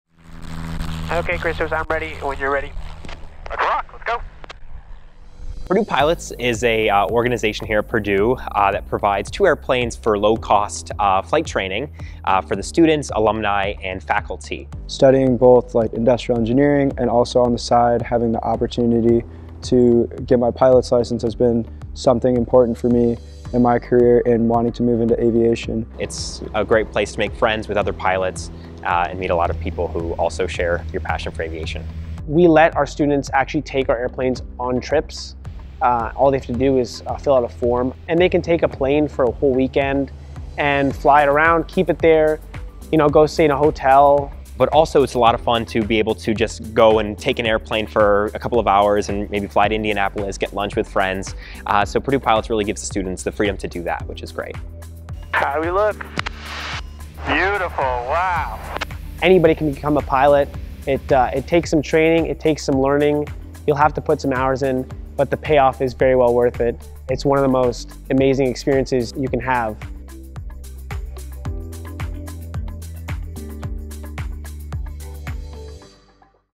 Taking Flight: Purdue Pilots
Purdue University is known for its innovation and opportunities for students to learn and grow. Created in 1956, Purdue Pilots Incorporated is just one of the many ways that Purdue fosters student involvement and innovation.

Purdue Pilots Inc. provides students of all majors with low-cost flight training and aircraft rentals. This allows all Purdue students the opportunity to take small steps and grow in their skills and knowledge about flight, mechanics and aircraft engineering.

By providing students the tools they need to be successful, Purdue is persistently working towards the next giant leap.